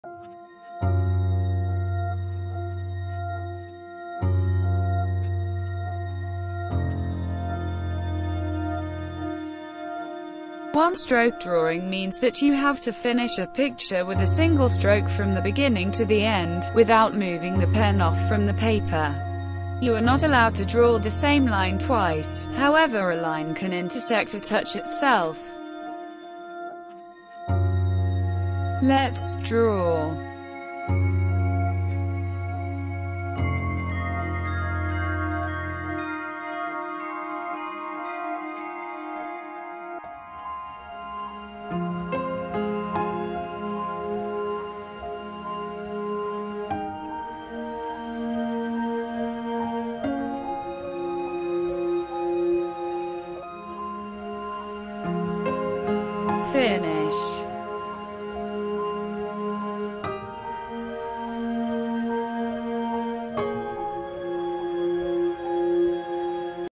One stroke WINDOW
Draw a picture below with a single stroke.

One stroke drawing means that you have to finish a picture with a single stroke from the beginning to the end, without moving the pen off from the paper.
You are not allowed to draw the same line twice, however a line can intersect or touch itself.